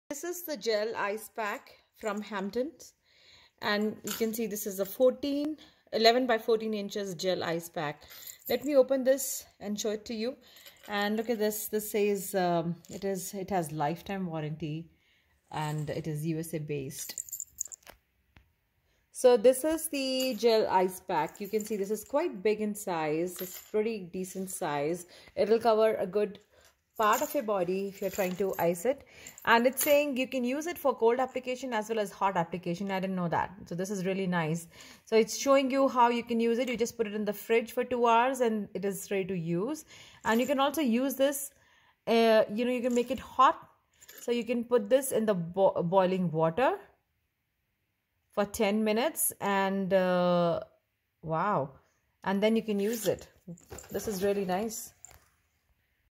Hampton Adams reusable ice pack hot cold therapy
Reusable Ice Pack for Injuries (Hot & Cold Therapy) Soft Gel, Flexible Medical Grade Compress - Refreezable Shoulder, Rotator Cuff, Knee, Perinatal, Neck, Headache Pain Relief, First Aid, Cryotherapy from Hampton Adams.This ice pack is quite decent in size so will cover a good part of your body.The ice pack can be used for ice or hot therapy .This is great for anyone dealing with sports injuries.